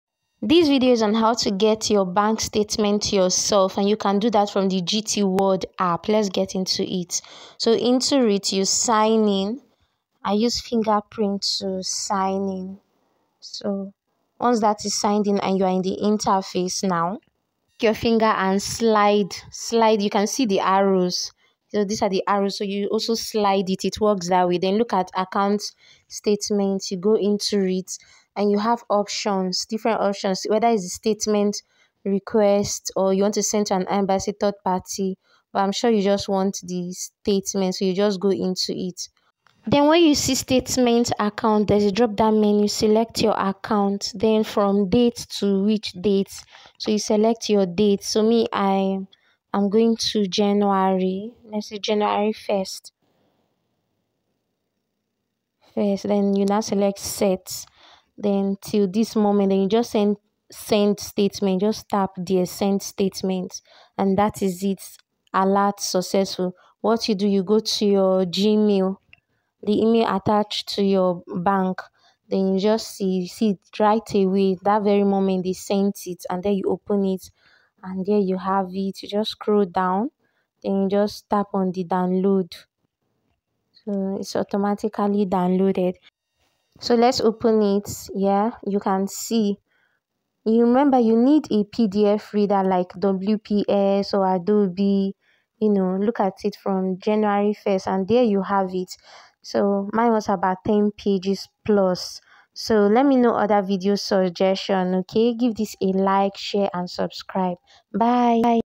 How download your GT Bank statement yourself from the GTWorld app
You don't need to go to the bank and stress yourself when you can get your GTB bank statement yourself that's why this video is on:

How to download your GT Bank statement yourself from the GTWorld app.

Guaranty Trust Bank has made it easy for customers to get their bank statements themselves through the GTworld app.